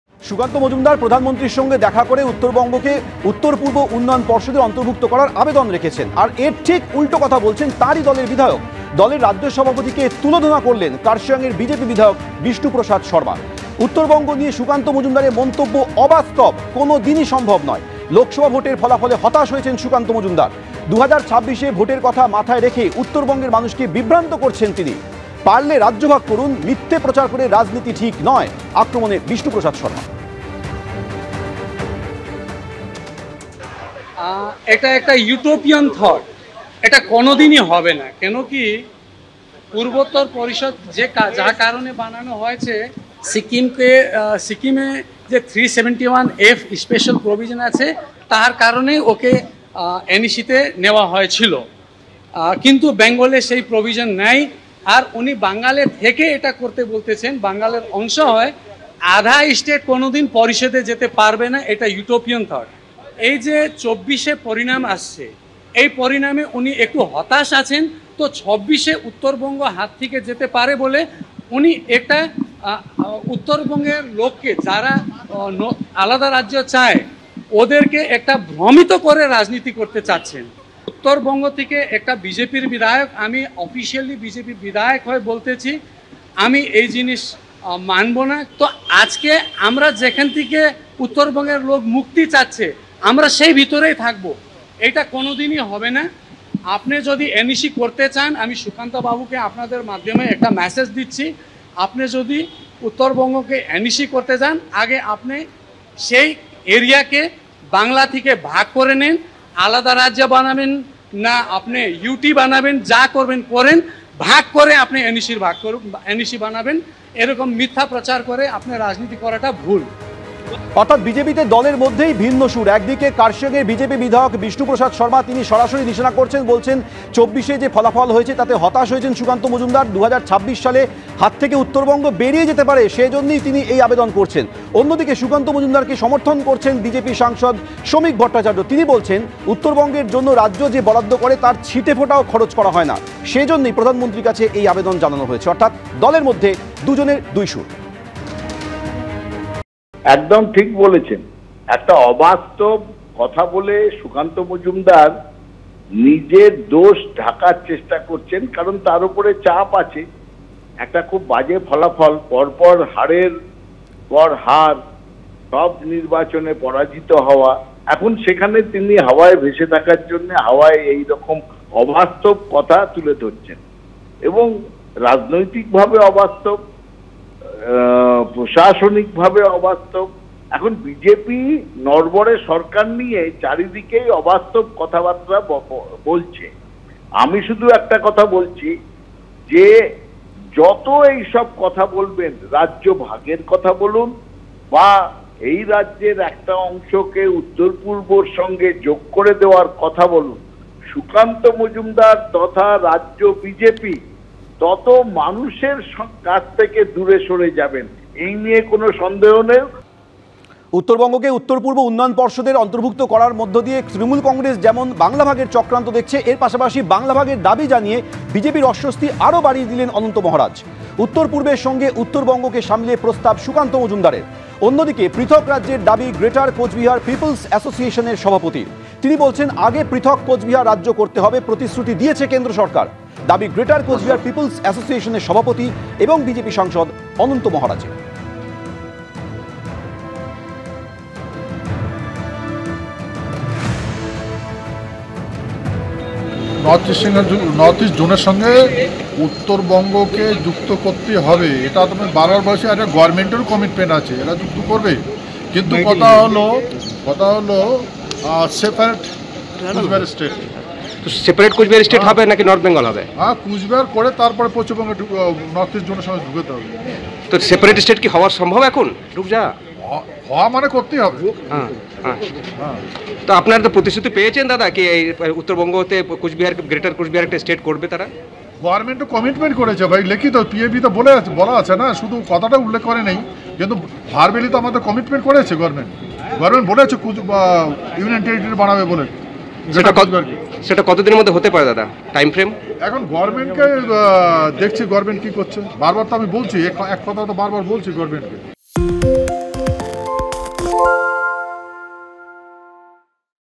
Sukanta Majumdar:'পারলে রাজ্য ভাগ করুন, মিথ্যে প্রচার করে রাজনীতি ঠিক নয়', বিধায়কের নিশানায় সুকান্ত
ABP Ananda LIVE: 'মানুষকে বিভ্রান্ত করছেন। পারলে রাজ্য ভাগ করুন, মিথ্যে প্রচার করে রাজনীতি ঠিক নয়', উত্তরবঙ্গ-মন্তব্যে দলেরই বিধায়কের নিশানায় সুকান্ত